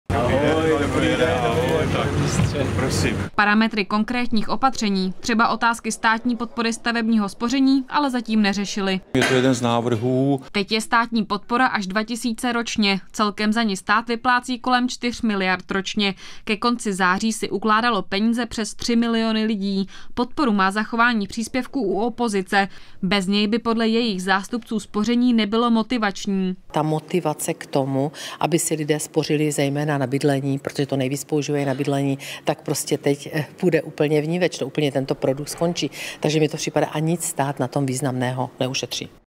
Neschopná vláda Petra Fialy zvažuje, že si tentokrát smlsne na mladých a jejich rodinách
Neschopná vláda Petra Fialy zvažuje, že si tentokrát smlsne na mladých a jejich rodinách. Chce totiž zrušit státní příspěvek na stavebko. Nic významného na tom stát neušetří, jen vezme lidem motivaci šetřit. Pusťte si video 👍